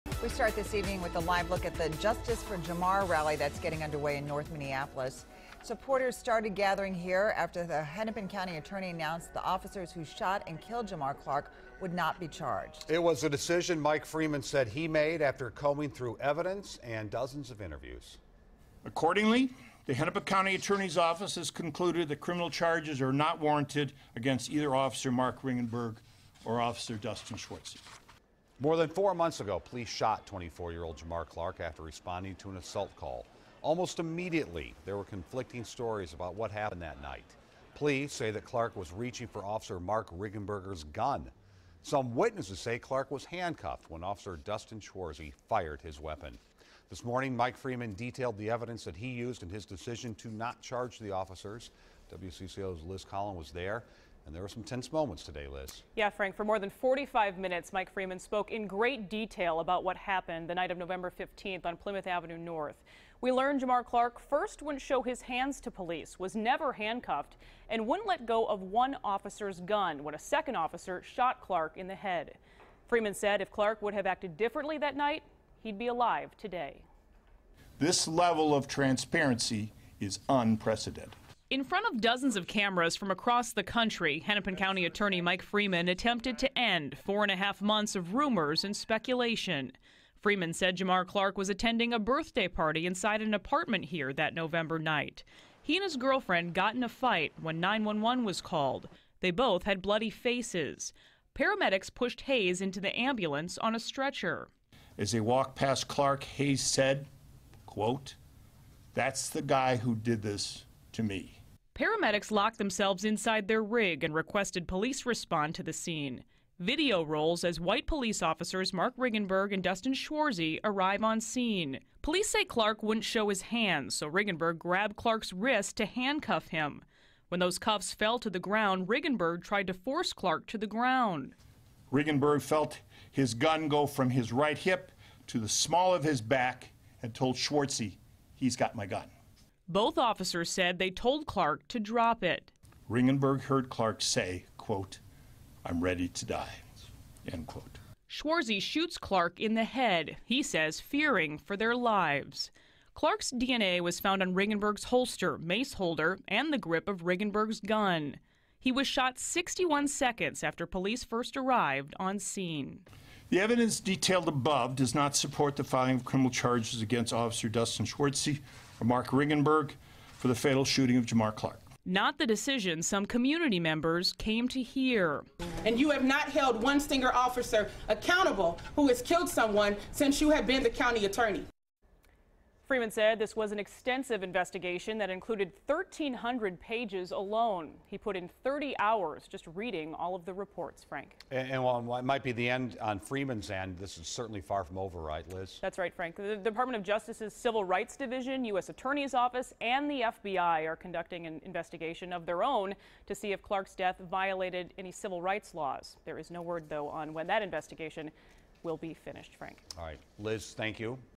Evidence Released In Jamar Clark Case
The Hennepin County attorney says Jamar Clark would be alive today if he would’ve acted differently last November, reports Liz Collin.
WCCO 4 News At 5 – March 30, 2016